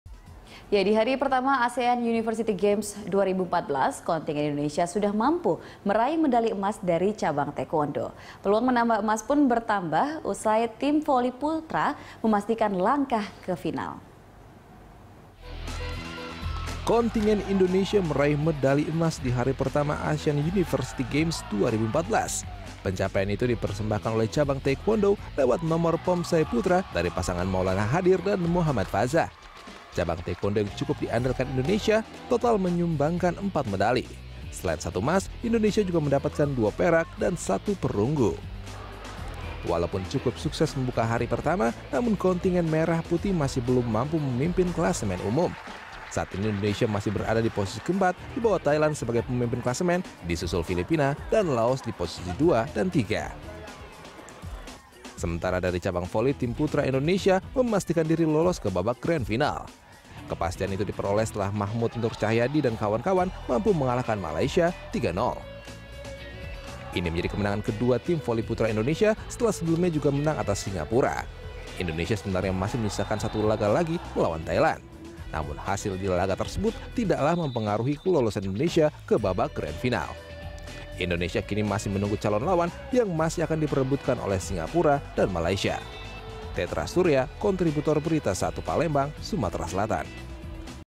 Indonesia Raih Emas ASEAN University Games Cabang Taekwondo
Di hari pertama ASEAN University Games 2014, kontingen Indonesia sudah mampu meraih medali emas dari cabang Taekwondo. Peluang menambah emas pun bertambah usai tim voli putra memastikan langkah ke final.

Facebook.com/BeritaSatuTV
Youtube.com/BeritaSatu